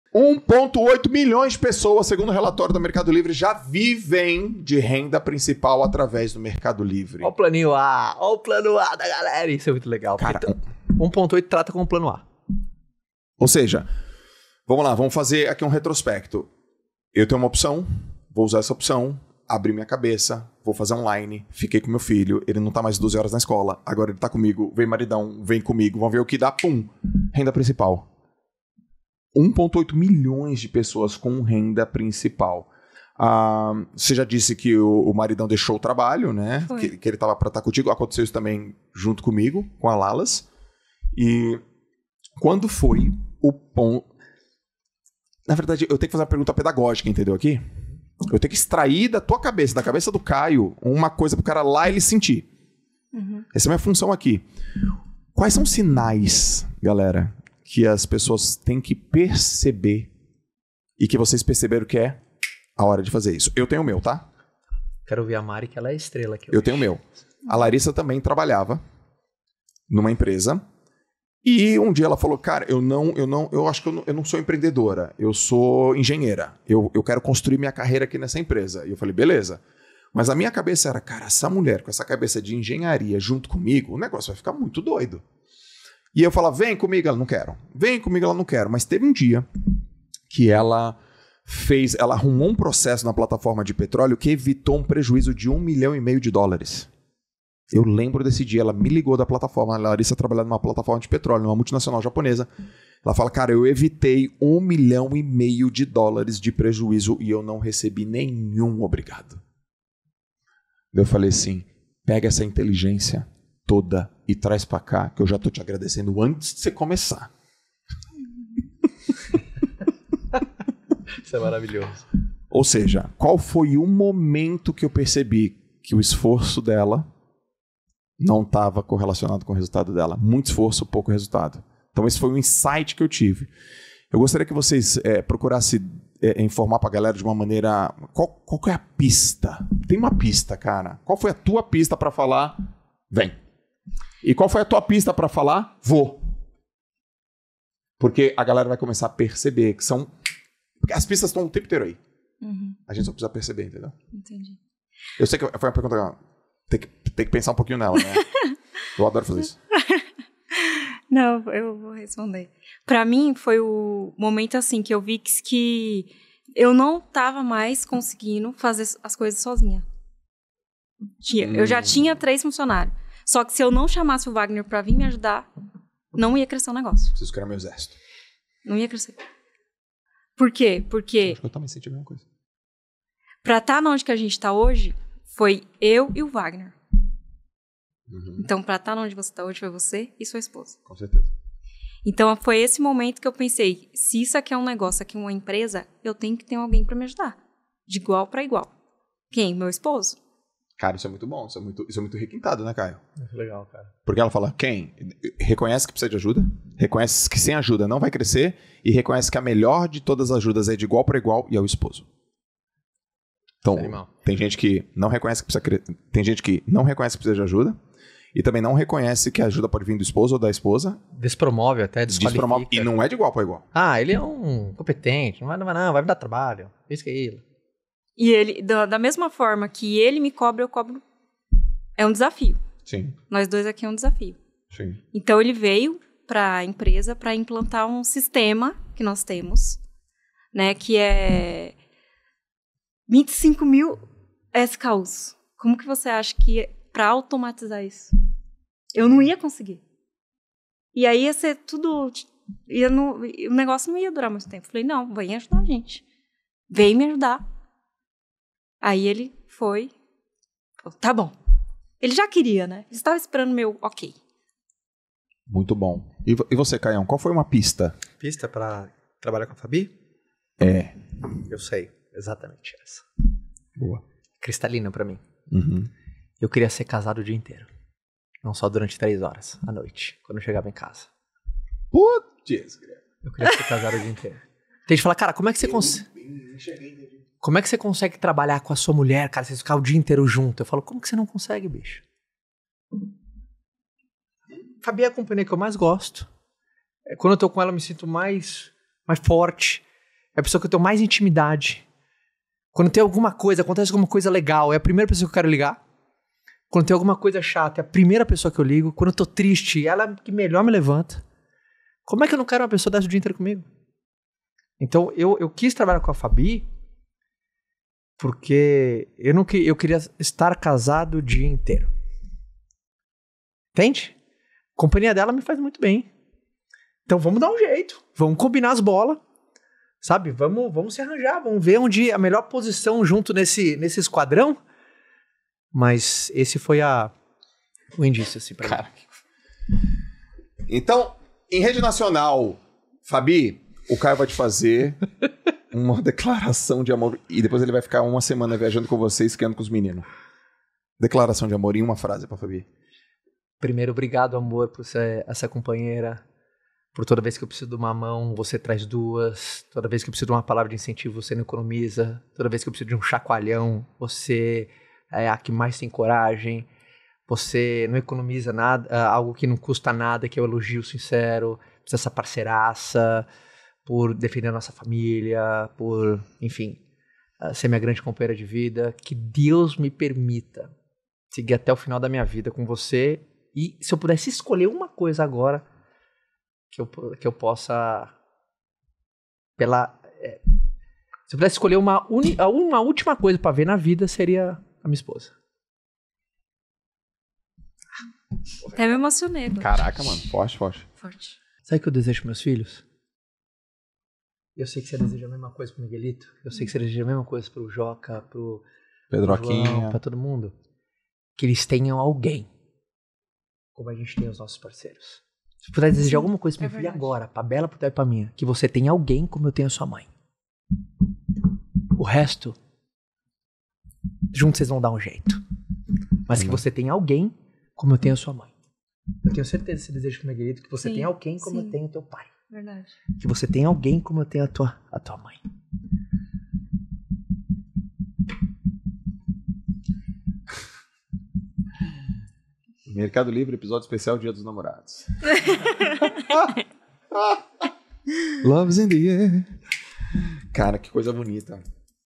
QUANDO é a hora IDEAL para EMPREENDER com seu PARCEIRO?
🎙️Host: Joel Jota

🗣️ Convidados: Caio Carneiro e Mariana dos Santos

/ joeljota
/ jotajotapodcast